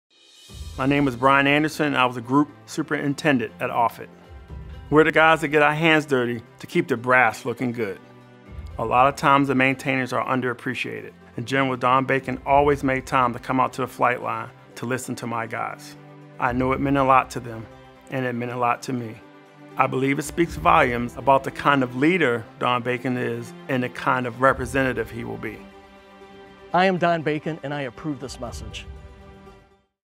Chief | Don Bacon for Congress
ABOUT THIS VIDEO:  Chief Master Sergeant Brian Anderson is an outstanding Airman and I greatly valued his leadership and counsel during my time as the Commander of Offutt.  Offutt Air Force Base has been lauded for its many successes over the years.  Behind all of these successes stand great patriots like Brian.

Text "DonBacon2016" to 313131 to contribute directly to Don's Campaign
*Message and data rates may apply.